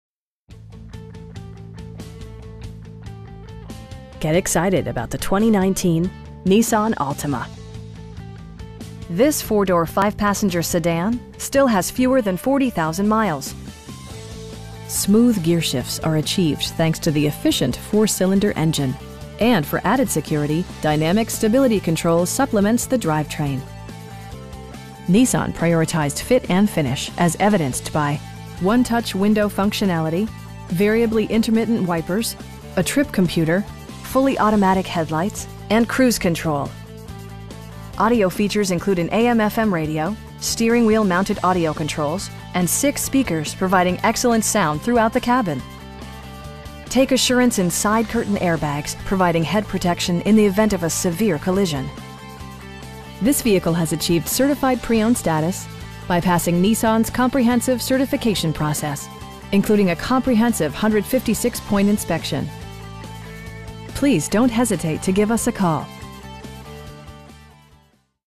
2019 Nissan Altima 2.5 S Sedan in Ames, IA 50010
Get excited about the 2019 Nissan Altima. This 4 door, 5 passenger sedan still has fewer than 40,000 miles! Smooth gearshifts are achieved thanks to the efficient 4 cylinder engine, and for added security, dynamic Stability Control supplements the drivetrain. Nissan prioritized fit and finish as evidenced by: 1-touch window functionality, variably intermittent wipers, a trip computer, front bucket seats, fully automatic headlights, an overhead console, and cruise control. Audio features include an AM/FM radio, steering wheel mounted audio controls, and 6 speakers, providing excellent sound throughout the cabin. Take assurance in side curtain airbags, providing head protection in the event of a severe collision. This vehicle has achieved Certified Pre-Owned status, by passing Nissan's comprehensive certification process, including a comprehensive 156 point inspection! We pride ourselves in consistently exceeding our customer's expectations. Please don't hesitate to give us a call.